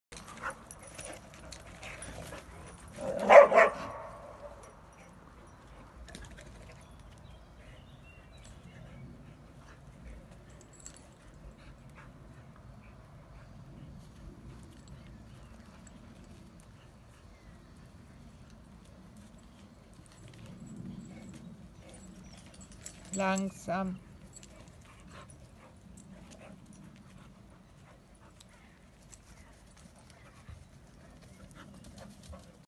Ronja (w) wartet auf ihrer Pflegestelle noch auf eine neue Familie
Ronja ist alterstypisch aktiv und verspielt. Natürlich muss ein Hund in diesem Alter noch eine Menge lernen, aber Ronja ist aufgeweckt und neugierig, sie wird den letzten Schliff schnell verinnerlicht haben.